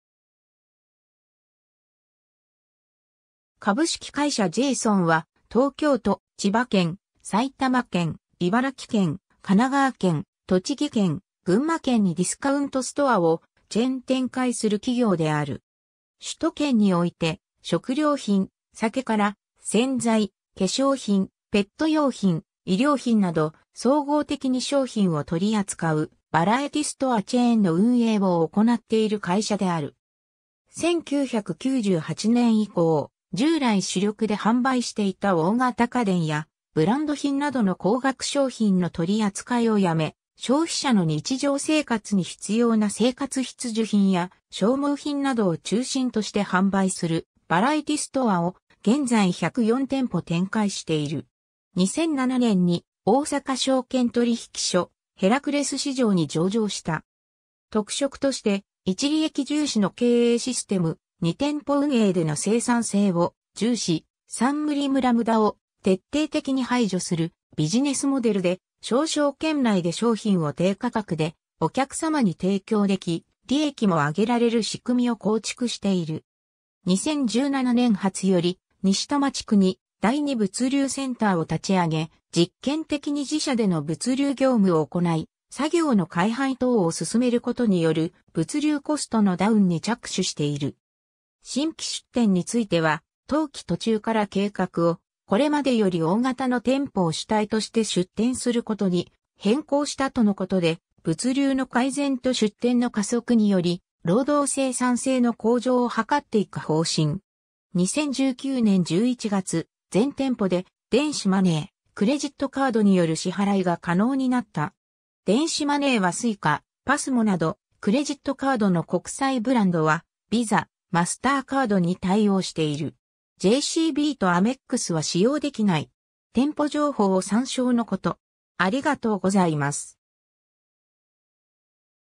ジェーソン
株式会社ジェーソンは、東京都・千葉県・埼玉県・茨城県・神奈川県・栃木県・群馬県にディスカウントストアをチェーン展開する企業である。
 首都圏において食料品・酒から、洗剤・化粧品、ペット用品・衣料品など総合的に商品を取り扱う「バラエティストア」チェーンの運営を行なっている会社である。
 1998年以降、従来主力で販売していた大型家電やブランド品などの高額商品の取り扱いをやめ、消費者の日常生活に必要な生活必需品や消耗品などを中心として販売する「バラエティストア」を現在104店舗（2017年10月時点）展開している。
 2007年に大阪証券取引所ヘラクレス市場（現　東京証券取引所ジャスタック市場）に上場した。
 特色として①利益重視の経営システム、②店舗運営での生産性を重視、③ムリ・ムラ・ムダを徹底的に排除するビジネスモデルで、小商圏内で商品を低価格でお客様に提供でき、利益も上げられる仕組みを構築している。
 2017年初より、西多摩地区に第2物流センターを立ち上げ、実験的に自社での物流業務を行い、作業の改廃等を進めることによる物流コストのダウンに着手している。
 新規出店については、当期途中から計画をこれまでより大型の店舗を主体として出店することに変更したとのことで、物流の改善と出店の加速により労働生産性の向上を図っていく方針。
 2019年11月、全店舗で電子マネー、クレジットカードによる支払いが可能になった。
電子マネーはSuica、PASMOなど、クレジットカードの国際ブランドはVISA、MasterCardに対応している。
JCBとアメックスは使用できない。
 「店舗情報」を参照のこと。